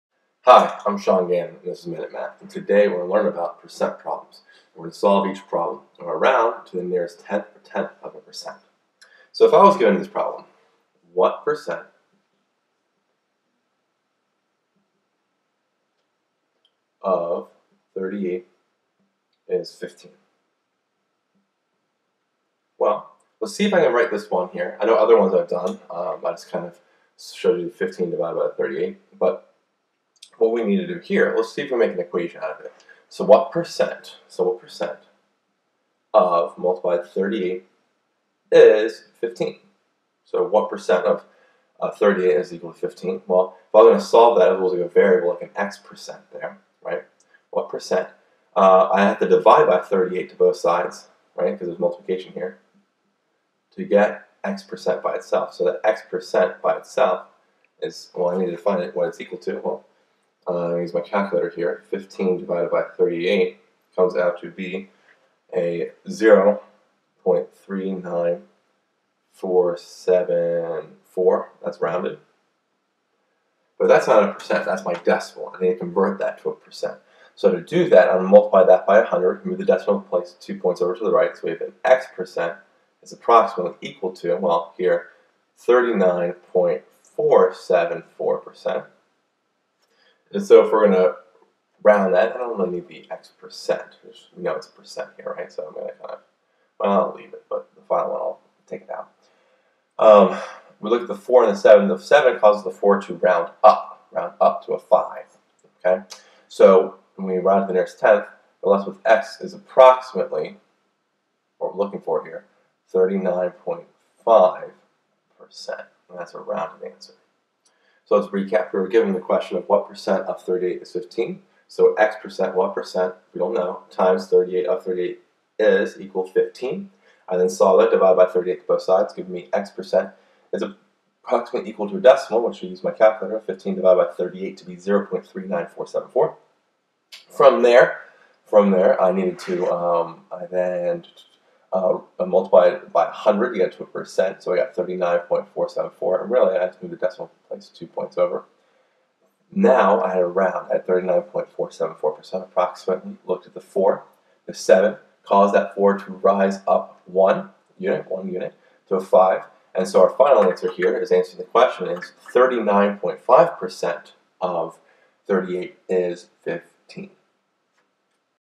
What percent of 38 is 15?
In this math video lesson on Percent Word Problems I answer the question: What percent of 38 is 15? This video discusses how to solve a percent word problem and is a good skill for Algebra 1.